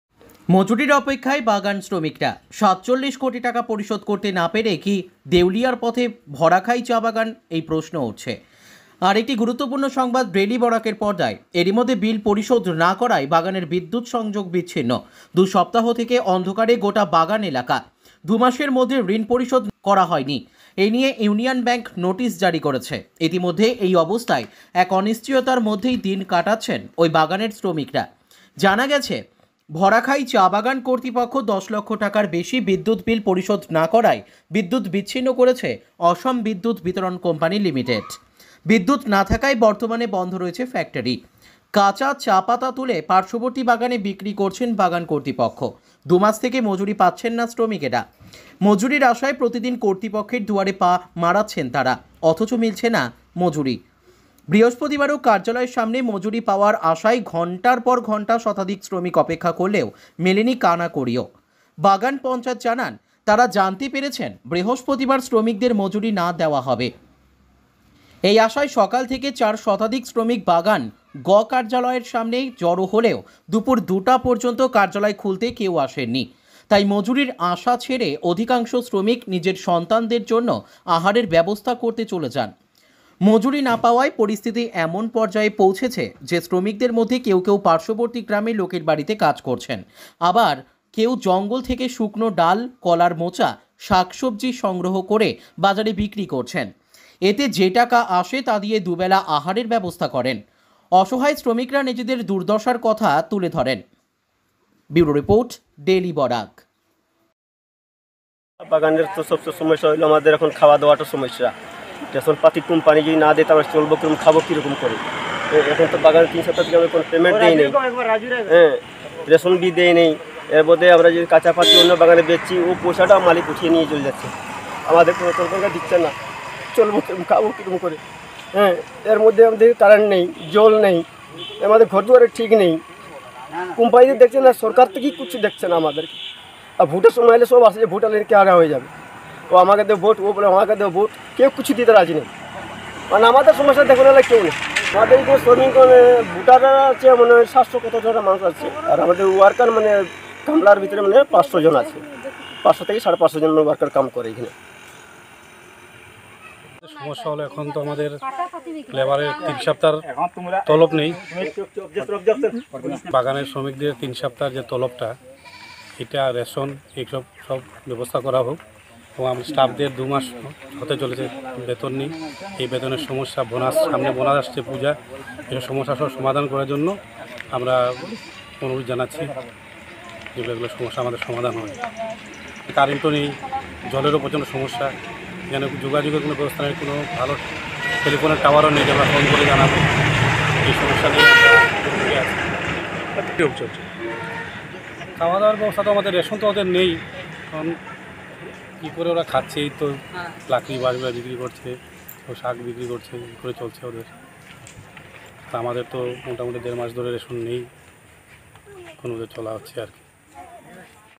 দুই সপ্তাহ ধরে বন্ধ ভরাখাই চা-বাগান, হাজিরা না পাওয়ায় বিপাকে শ্রমিকরা।
দুই সপ্তাহ ধরে বন্ধ ভরাখাই চা-বাগান, হাজিরা না পাওয়ায় বিপাকে চা শ্রমিকরা। বন্ধ হওয়ার পথে কি বাগান এই প্রশ্ন উঠছে।